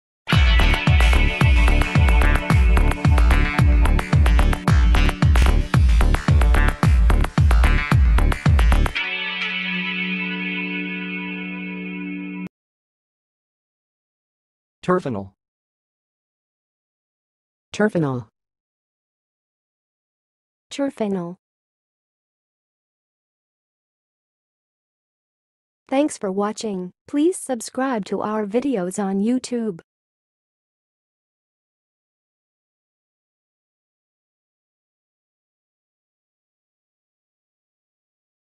How to say "terphenyl"! (High Quality Voices)
Watch in this video how to say and pronounce "terphenyl"!